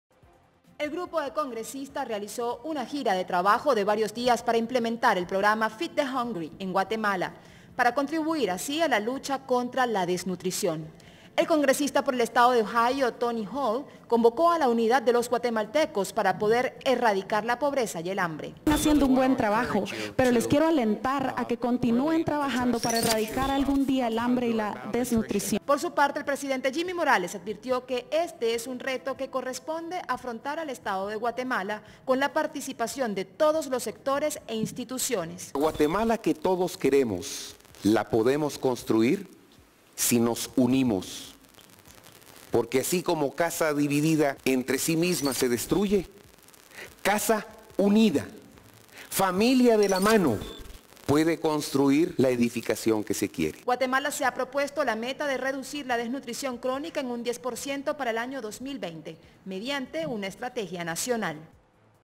Congresista EE.UU.: “Sería genial que Guatemala algún día elimine el hambre y desnutrición”
Congresistas estadounidense visitaron Guatemala y durante un encuentro con el presidente Jimmy Morales instaron al gobierno a trabajar para erradicar el hambre y la pobreza.

El grupo de congresistas realizó una gira de trabajo de varios días para implementar el programa "Feed the hungry" en Guatemala, contribuir así a la lucha contra la desnutrición.

El congresista por el estado de Ohio Tony Hall, convocó a la unidad de los guatemaltecos para poder erradicar la pobreza y el hambre.

Por su parte, el presidente Jimmy Morales advirtió que este es un reto que corresponde afrontar al estado de Guatemala, con la participación de todos los sectores e instituciones.

Guatemala se ha propuesto la meta de reducir la desnutrición crónica en un 10% para el año 2020, mediante una estrategia nacional.

Redacción